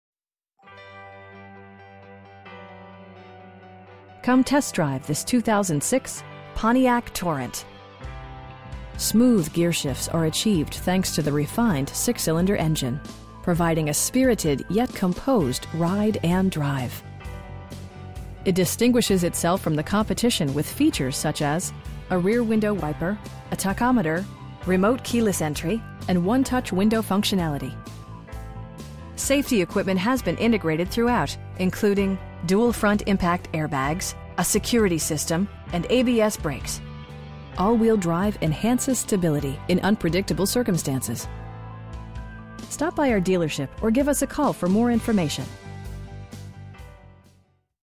2006 Pontiac Torrent near Flushing, Michigan
This vehicle is a 2006 Torrent with about 150K miles and is being powered by a 3. This 2006 Torrent is located near Flushing, Michigan.  4L V-6 cyl engine.  Some of the key features include: am/fm radio, air conditioning and cd player.  The black exterior combined with the ebony leather interior looks great together.  This SUV is being sold as-is without a warranty.  Some more key features on this Torrent include: remote keyless entry, power locks, hitch, moonroof, speakers: 6 and rear window wiper.  An AutoCheck Report is available upon request.
    Hank Graff Chevrolet Davison
    Address:
    800 N. State St.